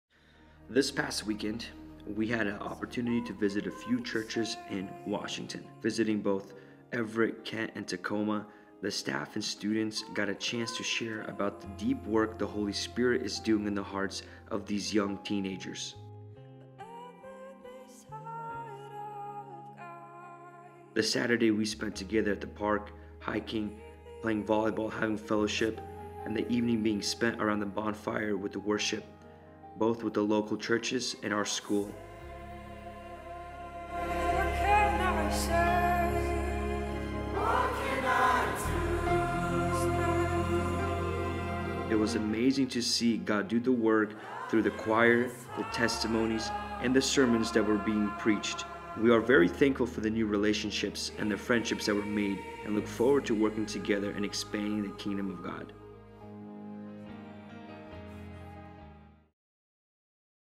Y4C 2019 Washington Visit - Recap
Great Commission Missionary School; Youth for Christ 2019 took a weekend trip north to Washington, visiting churches, meeting the youth and sharing of God has been doing in their lives.